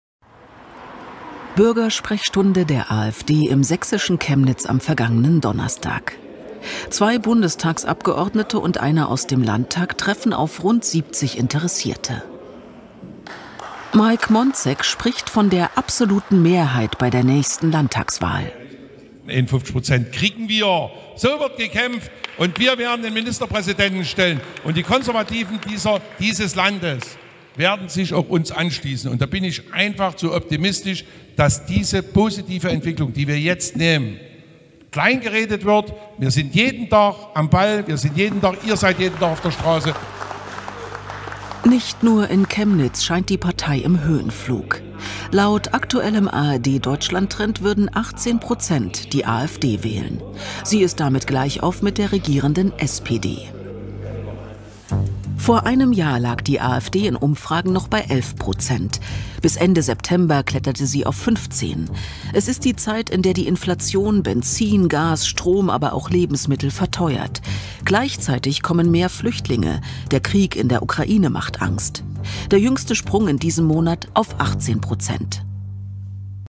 ARD: Nach den ganzen Zumutungen von CDU, SPD, Grüne, FDP ist die AfD im Umfragehoch? Volk zu doof?
Hmm, ob da die C-, Klima-, Migrations- und Russland-Hardliner von den Regierungsparteien selber daran schuld sind?

Nein, natürlich nicht, das Volk ist einfach zu doof, die klugen Entscheidungen aus Berlin zu verstehen?